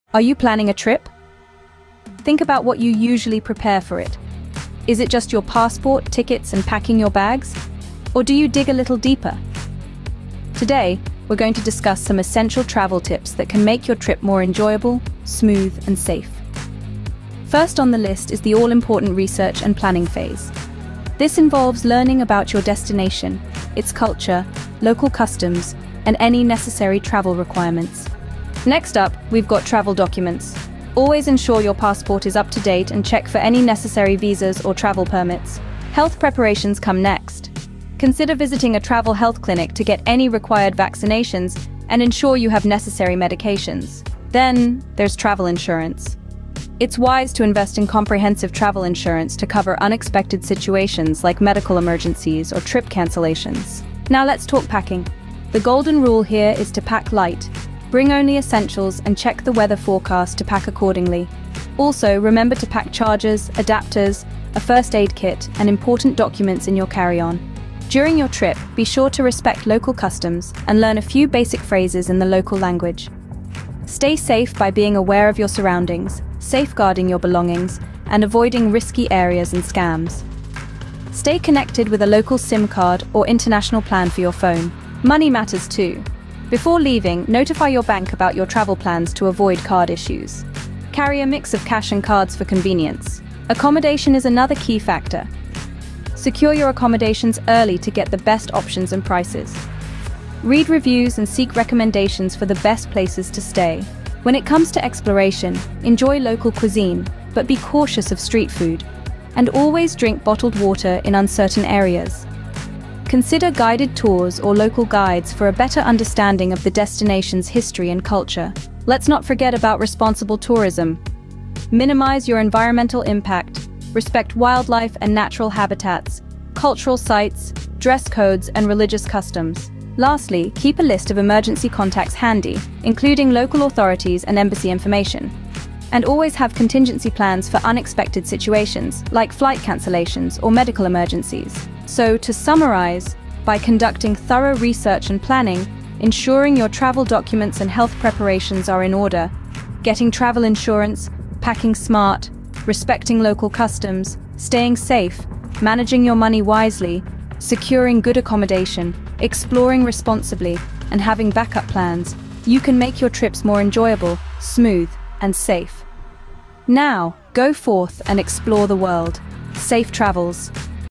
Travel Like a Pro! Essential Tips for Travelers
Are you planning a trip soon? 🌍✈️ Whether you're an experienced traveler or heading out for the first time, our latest video has got you covered! Join us as we share essential travel tips that will guarantee a smooth, enjoyable, and, most importantly, safe journey. From packing hacks to navigating through busy airports, we'll provide you with invaluable advice to make your trip stress-free. Bon voyage! 🧳🌟

OUTLINE:

00:00:00 Research and Planning
00:00:28 Travel Documents
00:00:35 Health Preparations
00:00:44 Travel Insurance
00:00:53 Packing
00:01:07 During the Trip
00:01:24 Money Matters
00:01:33 Accommodation
00:01:44 Exploration
00:01:58 Responsible Tourism
00:02:09 Emergency Preparedness
00:02:21 Summary and Conclusion